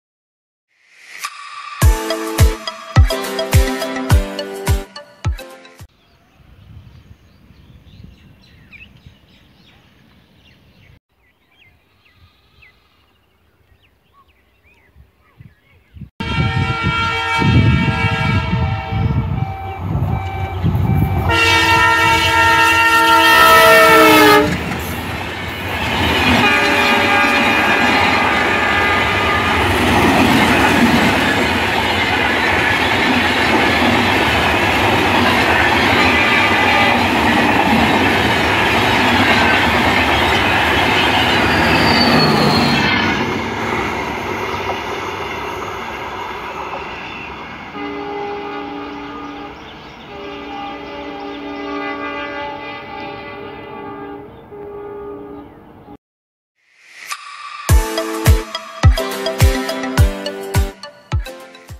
03060 Down New Alipurduar-Howrah Summer Special with Lal Kuan Rake is lead by WDM 3D||ER
Eastern Railways (ER)

03060 down new alipirduar-howrah summer special